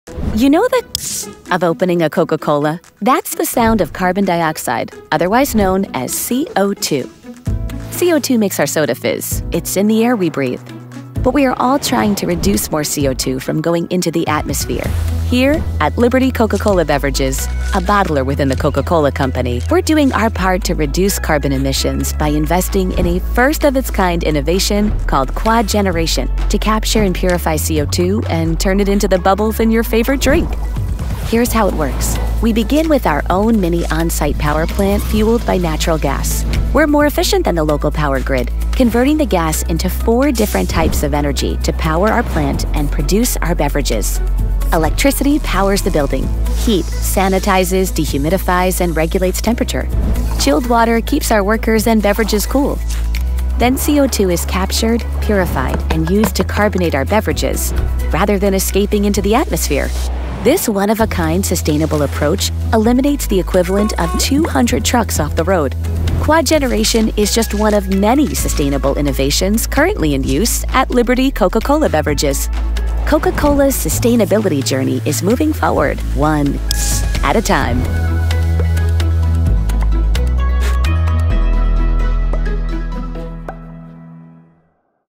Liberty Coca-Cola Introduces Quadgeneration Plant in Elmsford, NY
Liberty's Elmsford facility is thrilled to introduce the Quadgeneration Plant, a first of its kind in the country to independently generate electricity, heat, cooling, and recover carbon dioxide to convert it to beverage-grade use. The Quadgeneration Plant is paving the way for the future of sustainable manufacturing and bottling.